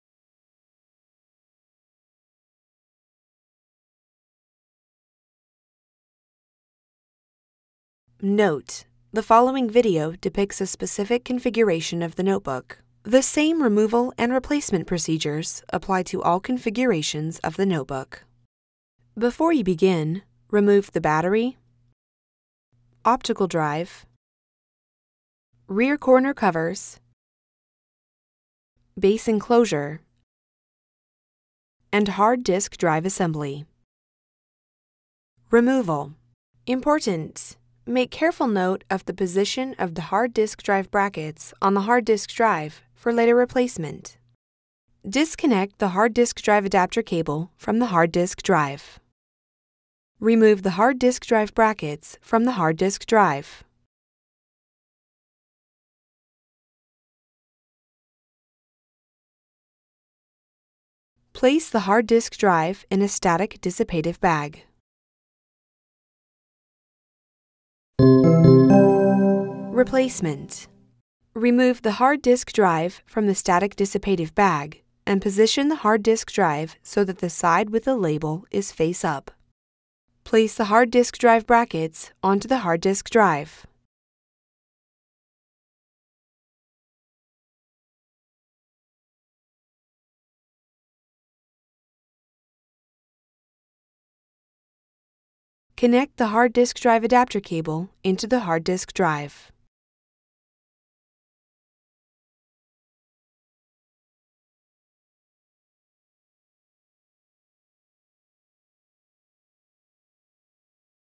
Replace the Hard Disk Drive Brackets | Pavilion 15-ab000 Notebook | HP Support
Learn how to remove and replace the hard disk drive brackets on the Pavilion 15-ab000 notebook